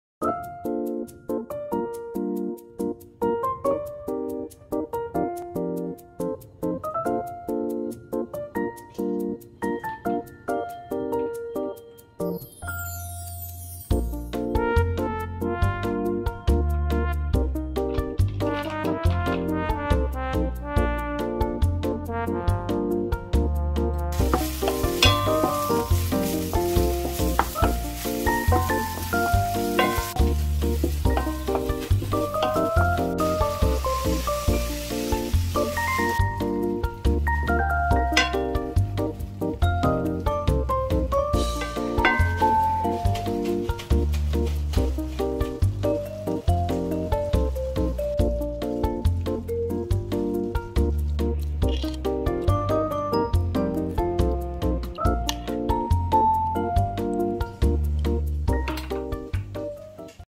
새송이버섯볶음&들깨무나물 | 아이반찬으로 좋아요
아이반찬으로 좋은 새송이버섯볶음&들깨무나물 만들었어요 :)